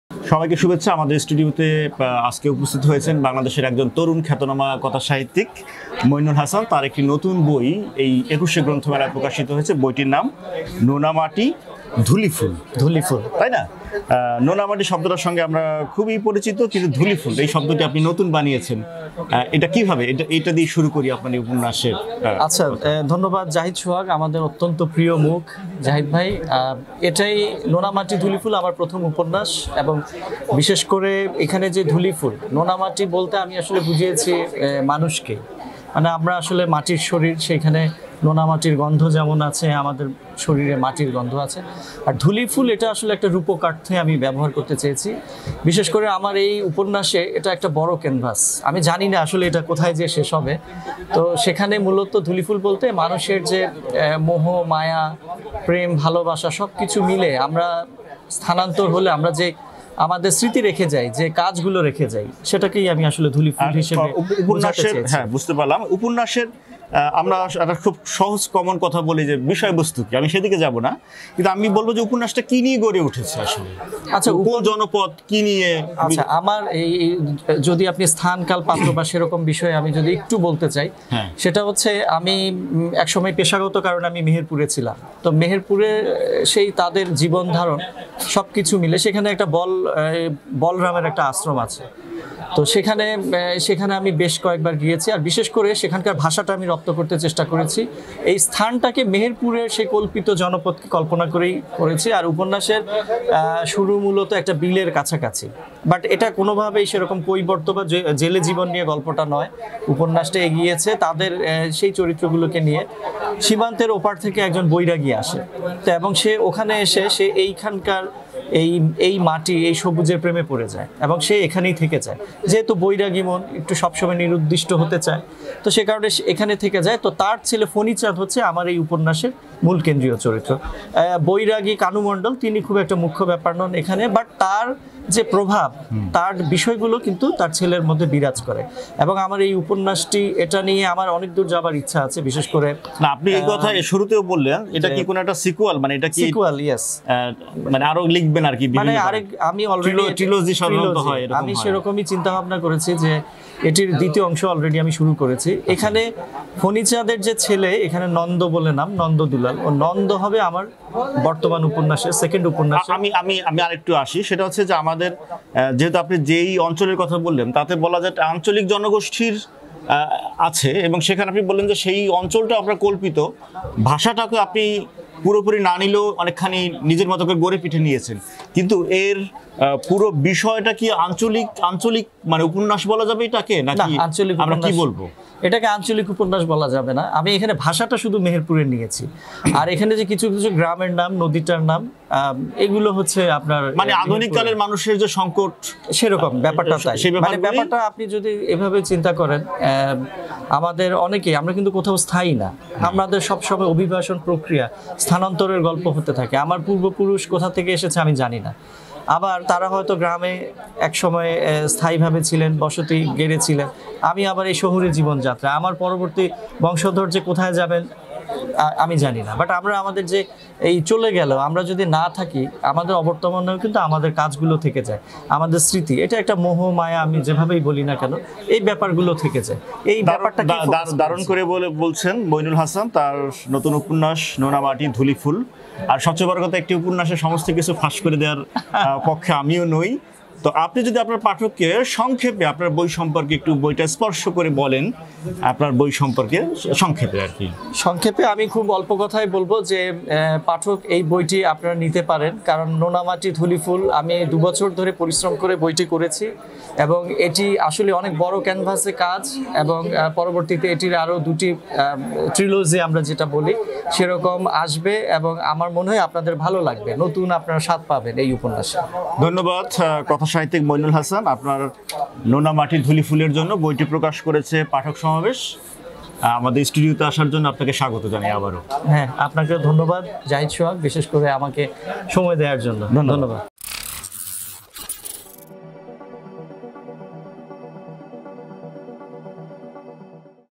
মঈনুল হাসানের ‘নোনা মাটি ধূলিফুল’।। বই নিয়ে কথা
সাহিত্য বিষয়ে বাংলা ট্রিবিউনের আয়োজন ‘বই নিয়ে কথা’। এবার কথাসাহিত্যিক মঈনুল হাসান নিজের উপন্যাস ‘নোনা মাটি ধূলিফুল’ নিয়ে কথা বলেছেন। সঞ্চালনায় জাহিদ সোহাগ।